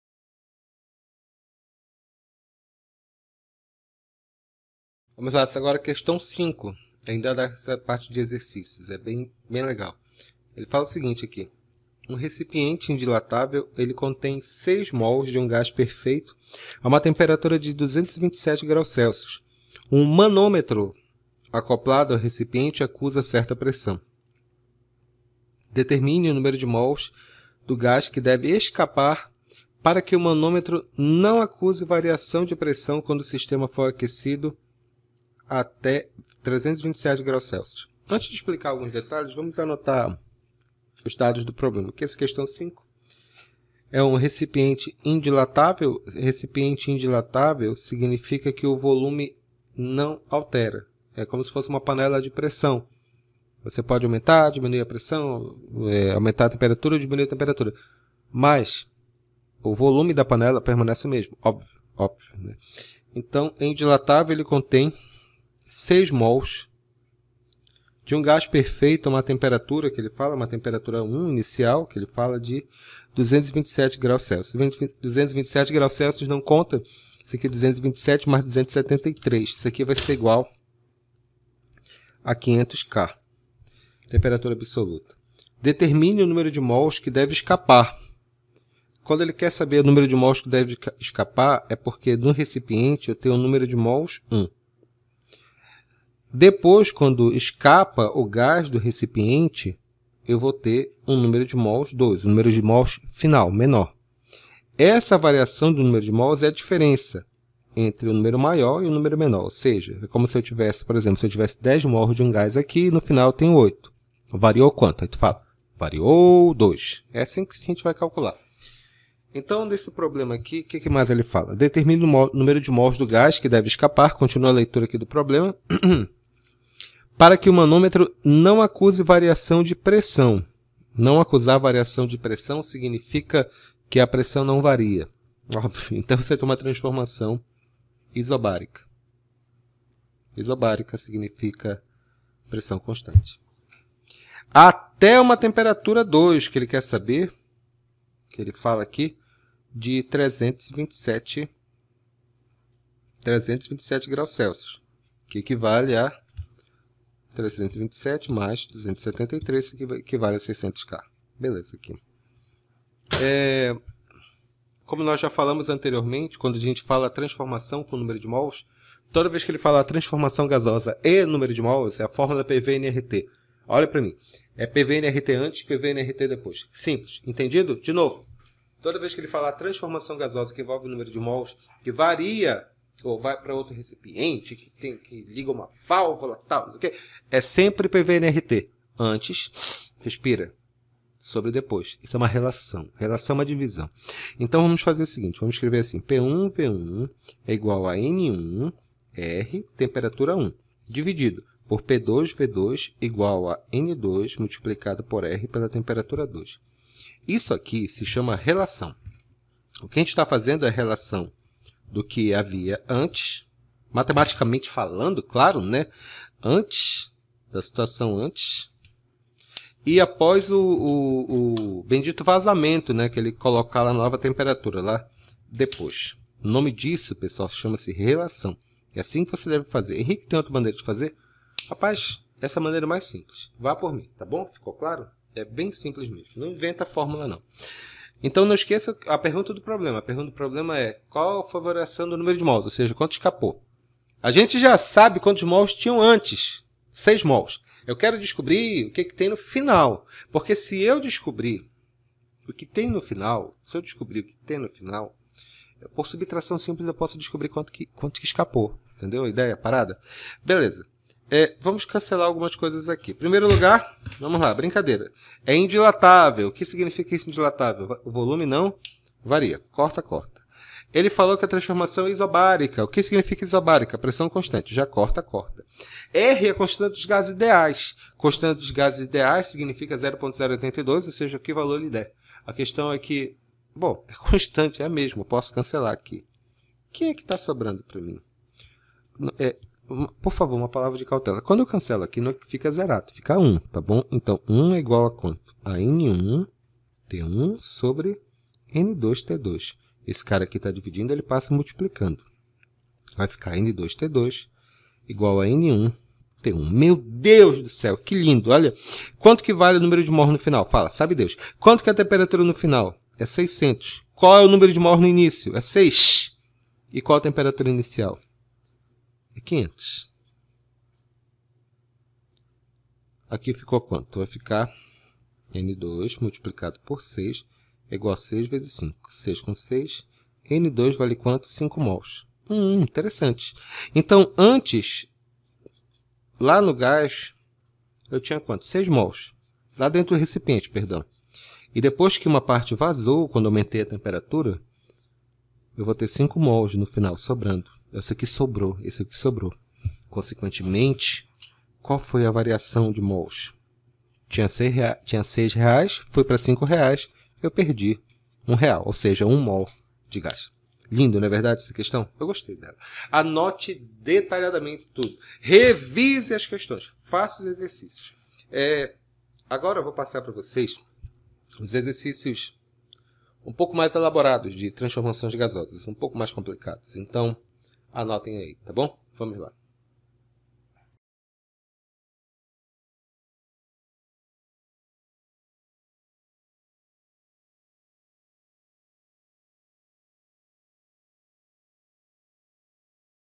Estudo dos Gases - Parte 9 - Clapeyron e Equação Geral dos Gases - Exercícios Comentados (C)
Aula do Professor Henrique Litaiff

Estudos dos Gases - Parte 9 - Exercícios comentados.